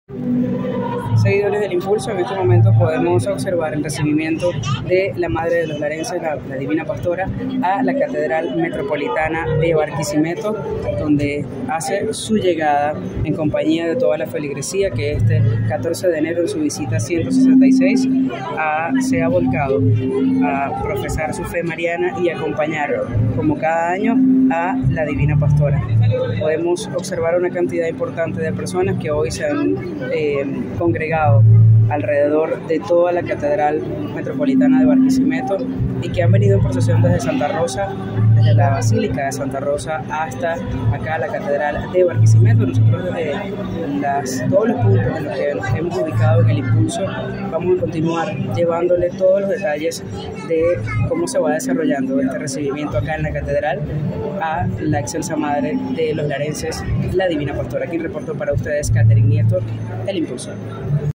Miles de fieles reciben a la Divina Pastora en la Catedral de Barquisimeto
Este domingo 14 de enero, la ciudad de Barquisimeto se vistió de fiesta para recibir a la Madre de los larenses, la Divina Pastora, en su visita número 166. La imagen de la Virgen llegó a la Catedral Metropolitana acompañada de una multitud de devotos que le expresaron su amor y fe.

La procesión de la Divina Pastora comenzó en la Basílica de Santa Rosa, y recorrió las calles de la ciudad hasta llegar a la Catedral, donde fue recibida con aplausos, cantos y oraciones. Los fieles le ofrecieron flores, velas, pañuelos y otros objetos de devoción, así como también le pidieron favores, milagros y protección.

Texto: Eduardo Soteldo
Reporte: Katherine Nieto